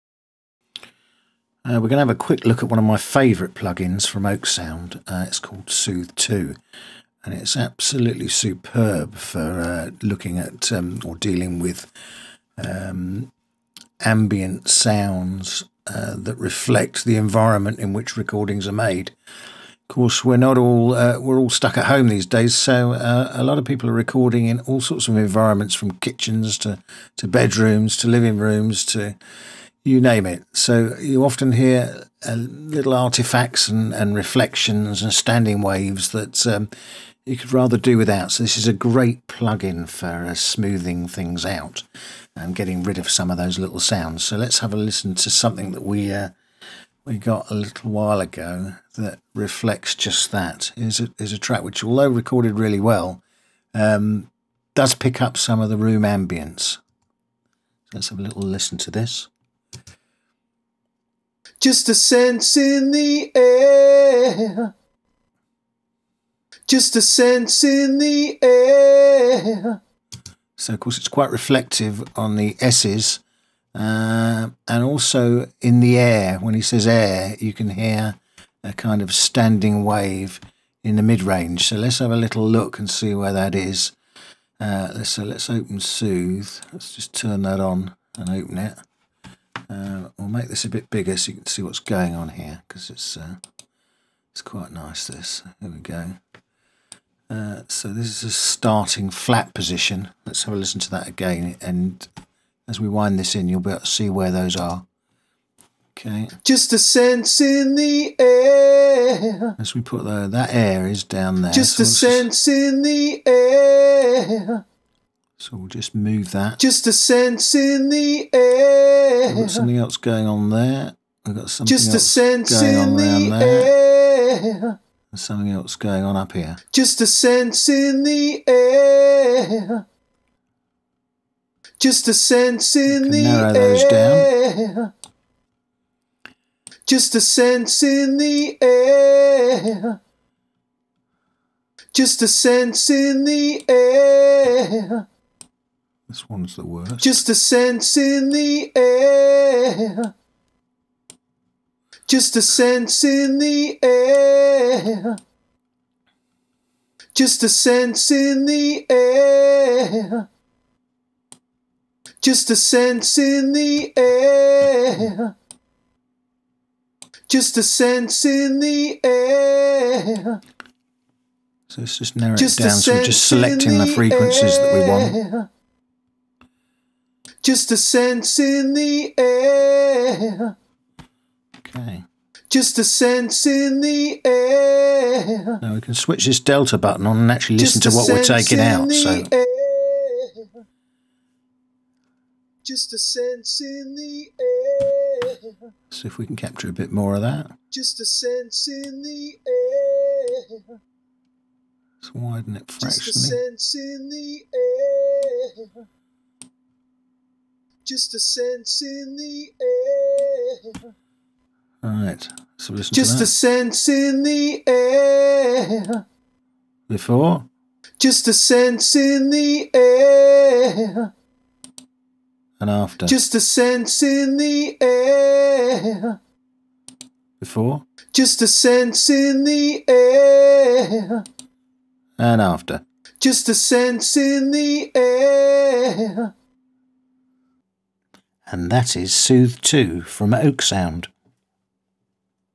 How To Eliminate Ambient Sounds Using Oeksound Soothe 2
In this Spotlight Review we use oeksound's Soothe 2 plugin to show how you can easily eliminate some of the ambient sounds that can creep into a vocal recording.